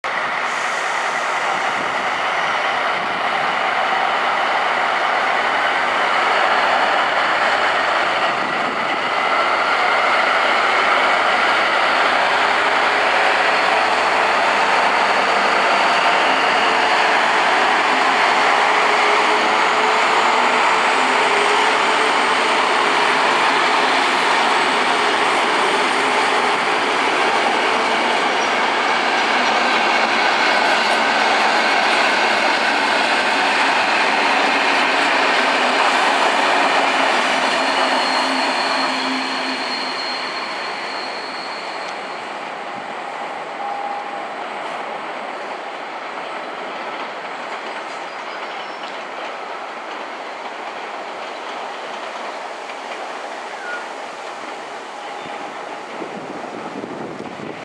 Brejlovci 750 702+703 Doudleby nad Orlici
Přípřež brejlovců odjíždí s R791 do Letohradu (17min spoždění:D)